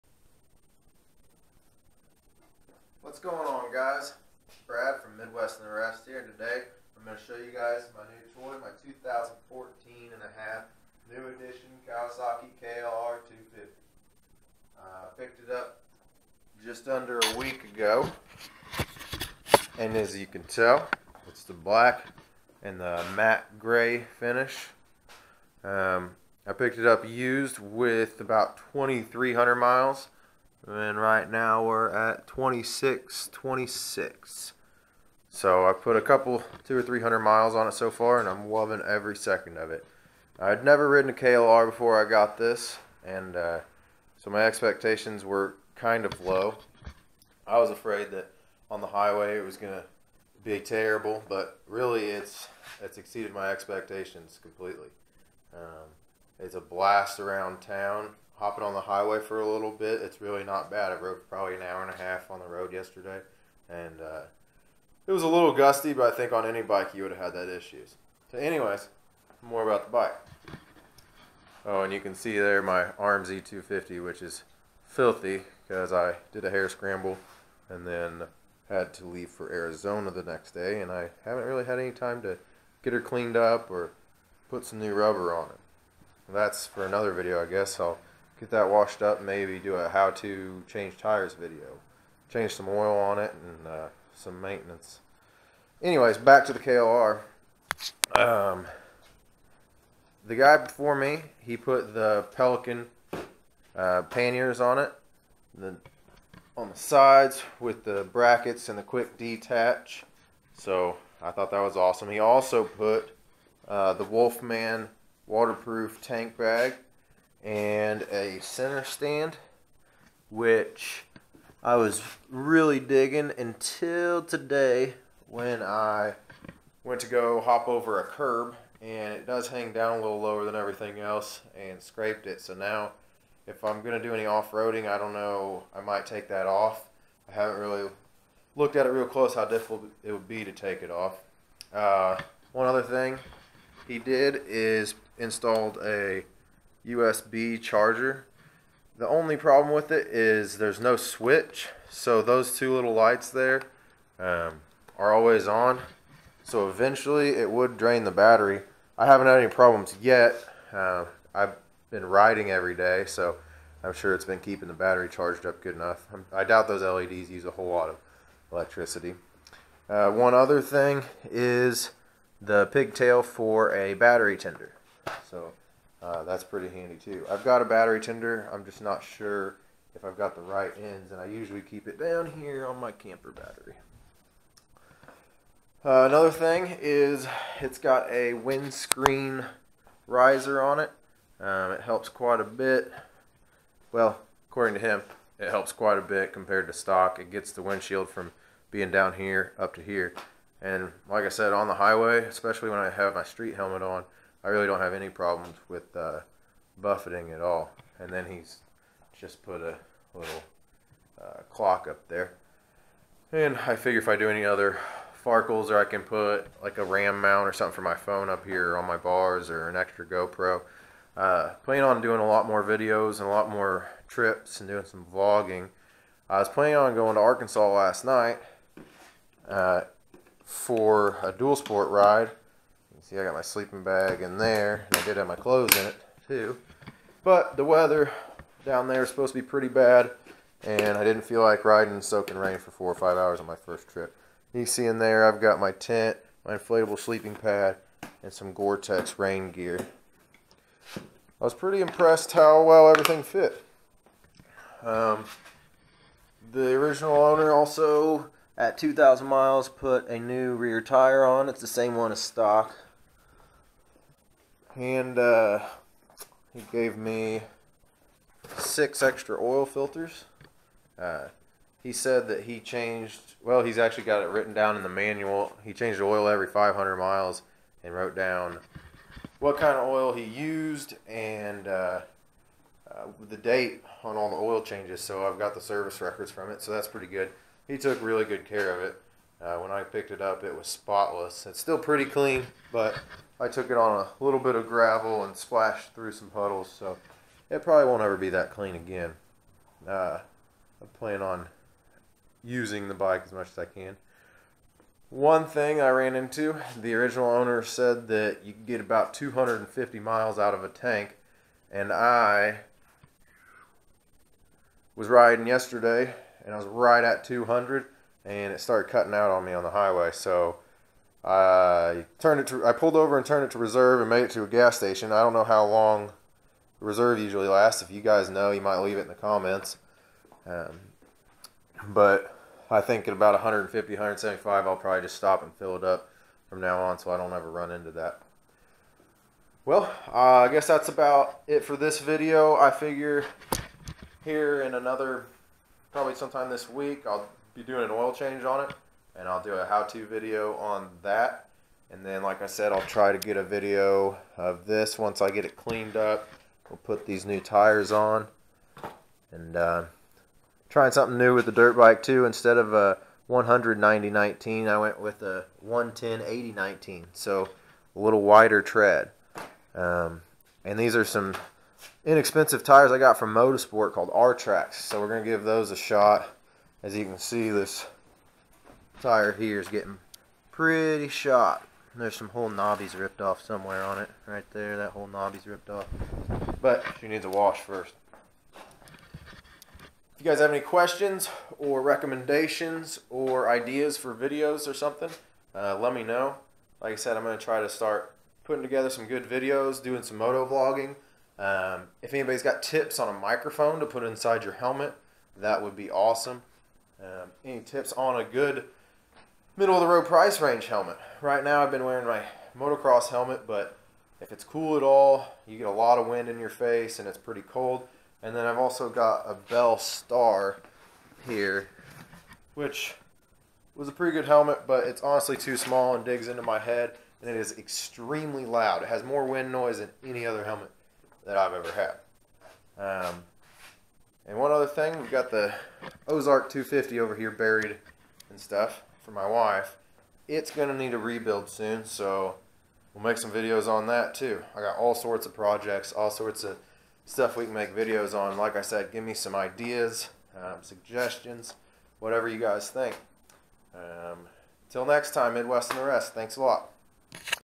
MotoVlog#1 My New KLR 650! Farkles and Mods so far...Pelican Panniers
My dreams came true!  After countless hours of browsing craigslist and swap and shops, I found the perfect platform for my adventure/dual sport adventures.

In this video I give a quick overview of the bike and the farkles and mods put on by the previous owner and discuss some of my future plans with the bike.